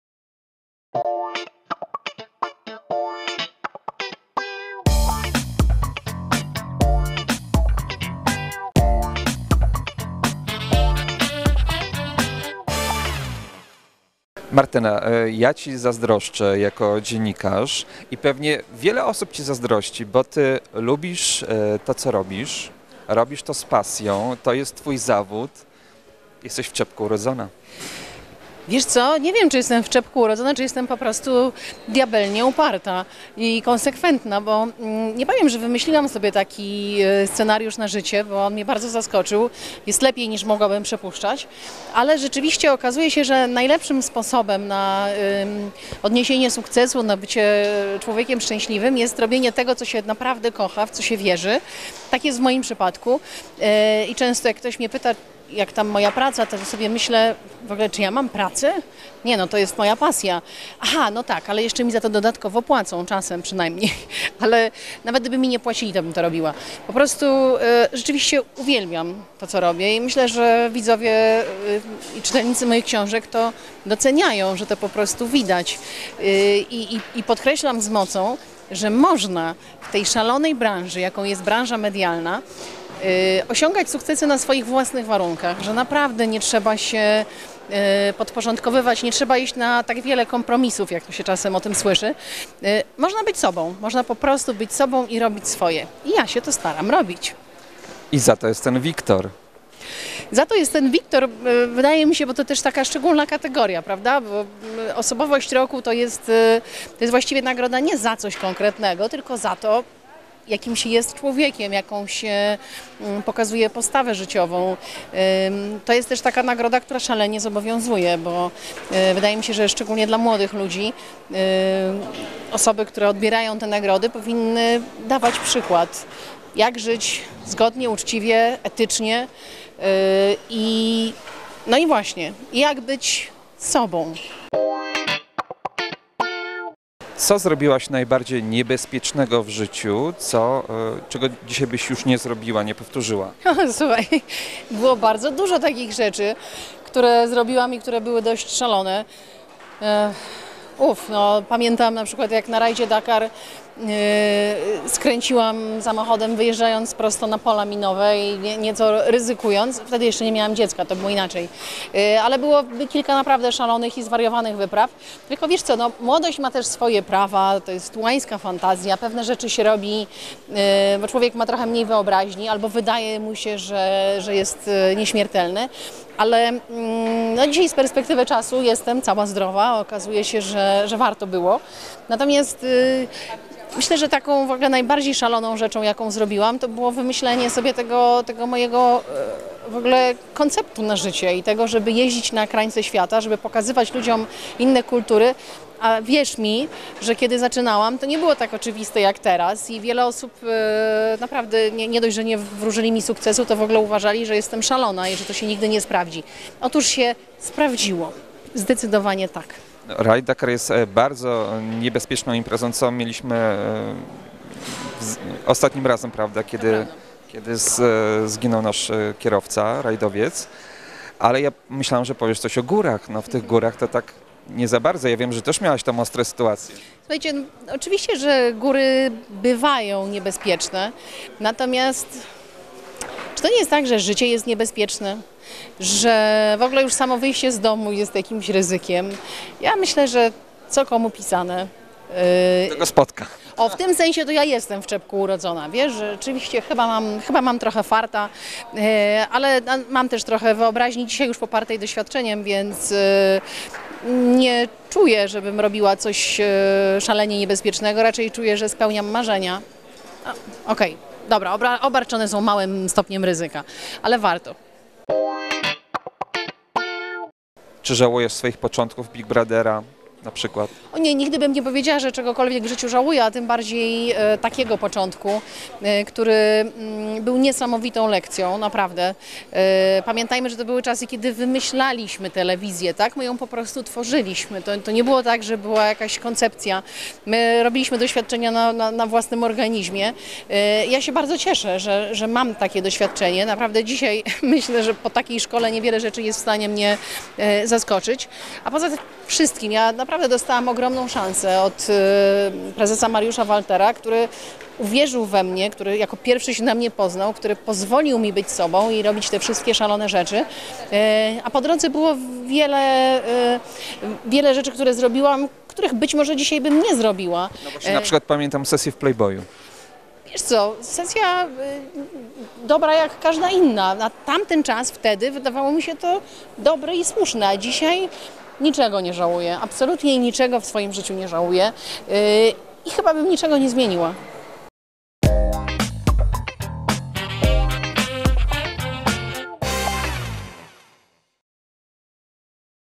Martyna Wojciechowska otarła się o śmierć | Wideoportal
Dziennikarka zapytana przez Roberta Patoletę o najniebezpieczniejszą sytuację podczas swoich wypraw wyznała, że w trakcie rajdu Dakar wjechała na pole minowe. - Na szczęście nie miałam jeszcze córki - dodała. Laureatka Wiktora opowiedziała też o swoim sposobie na sukces, "Big Brotherze" oraz sesji w "Playboyu".

TOP 20